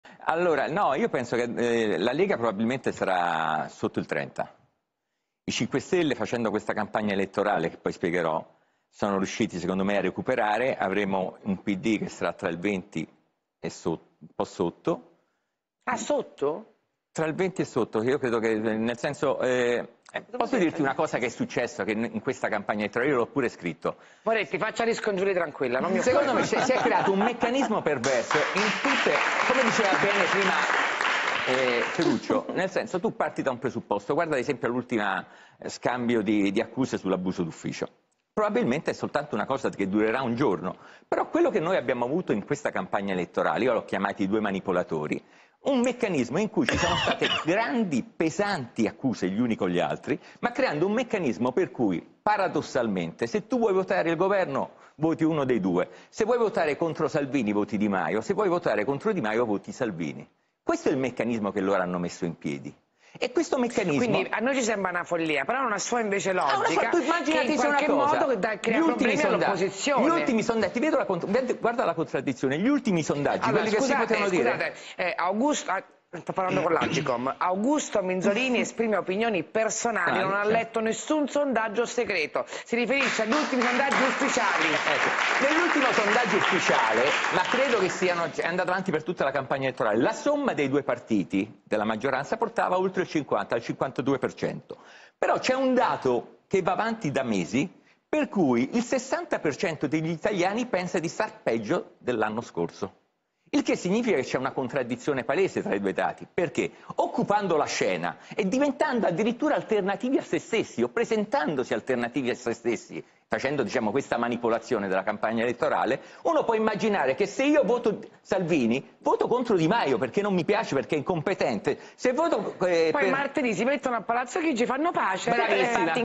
Augusto Minzolini: 'Prevedo la Lega sotto il 30, il M5S recupera contro Salvini e il PD sotto il 20'
Le previsioni elettorali, strettamente personali, dell'ex direttore del Tg1 e la strana campagna elettorale per le Europee